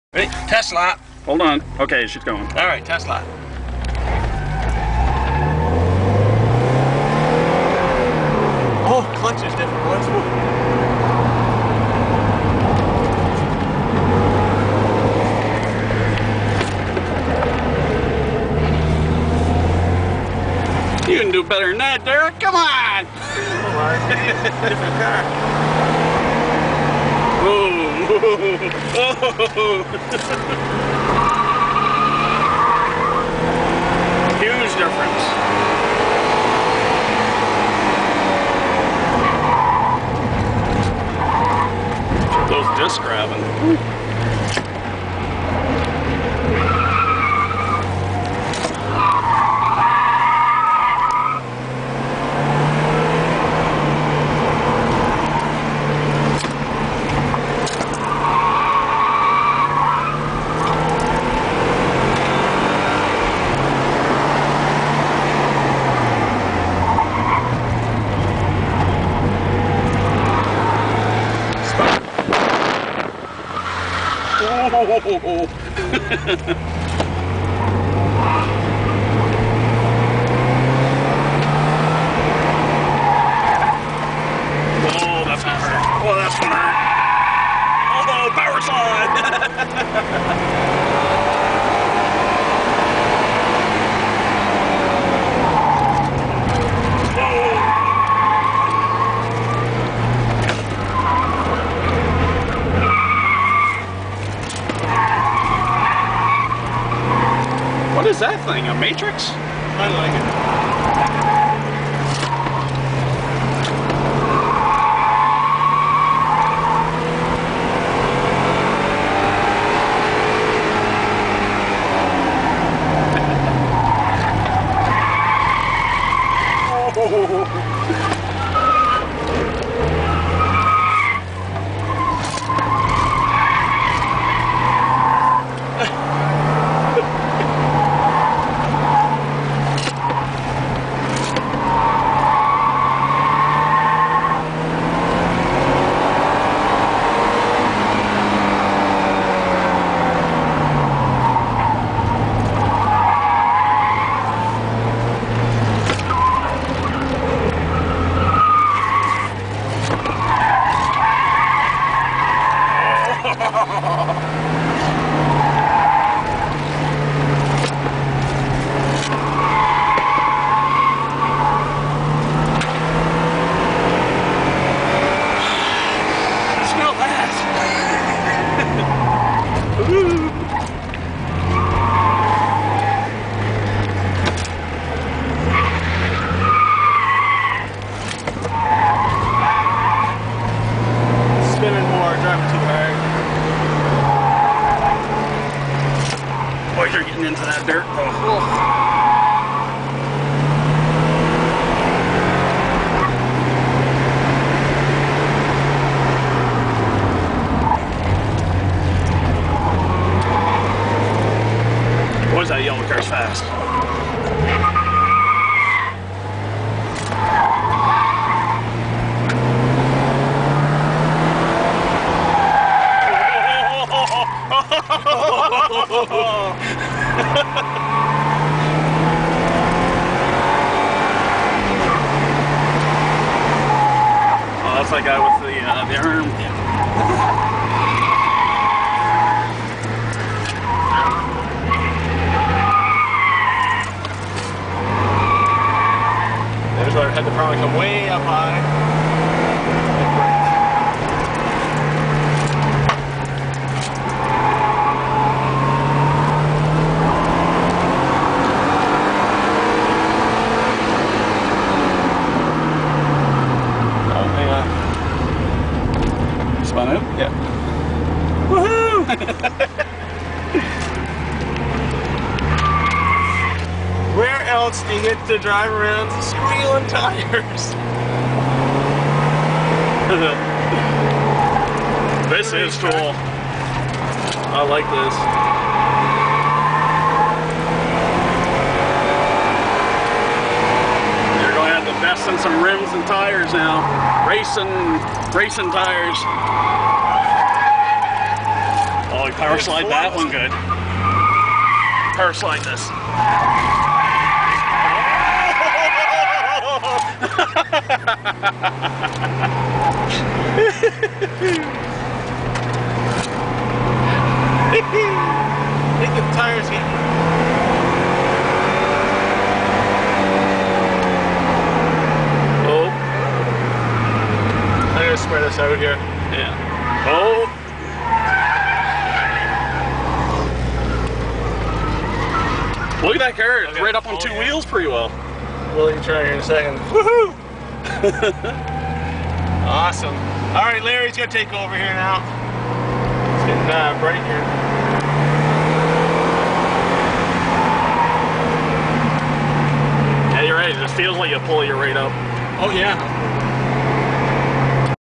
Porsche at Speedway 660
This video was taken by Larry Fairweather. Derek Gullison is driving a 1997 Porsche 944S at Speedway 660 on their small in track road course.